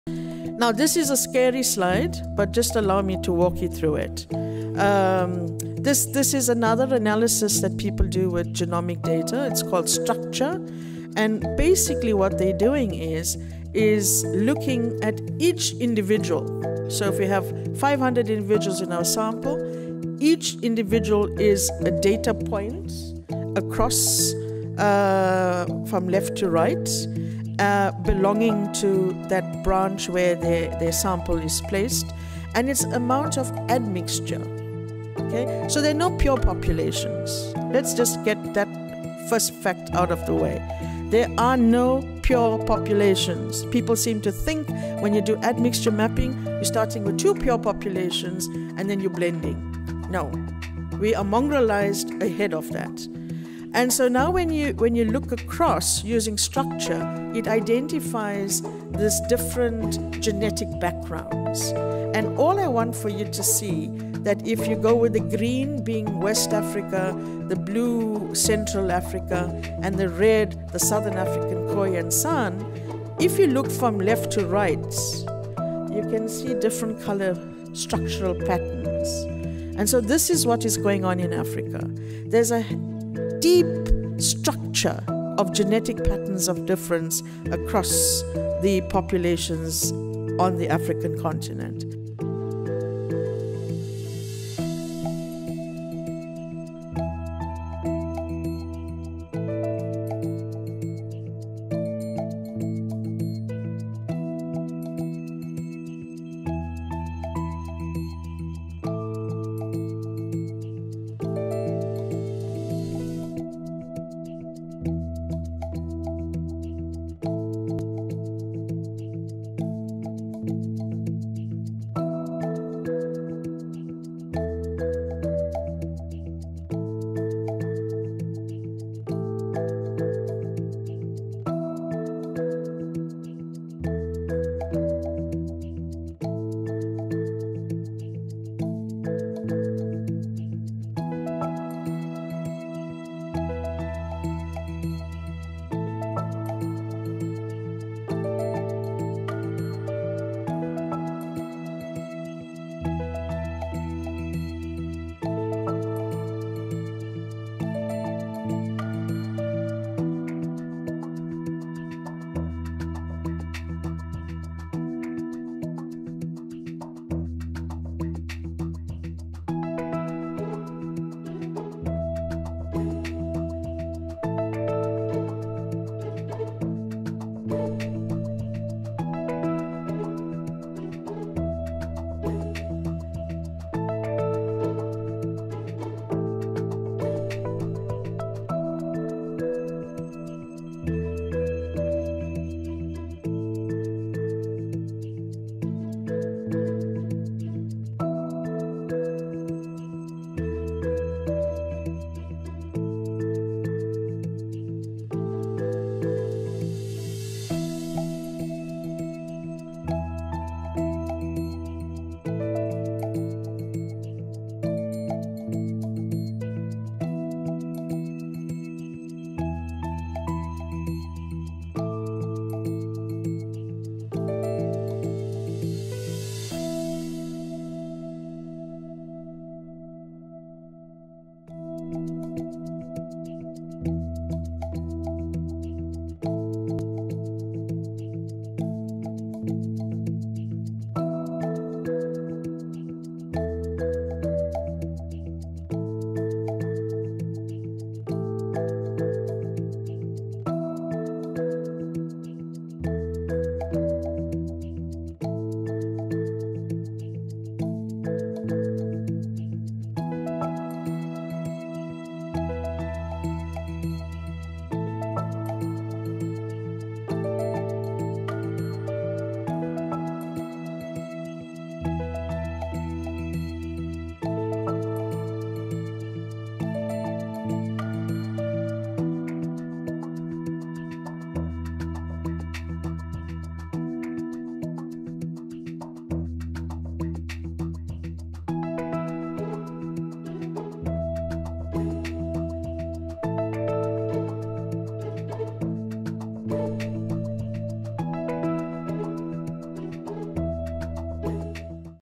Genetic Pioneer Professor Himladevi Soodyall
Professor Himladevi Soodyall is a trailblazing South African geneticist born in Durban in 1963. Her groundbreaking research has unveiled the oldest DNA in living individuals, supporting the theory of human evolution originating in southern Africa.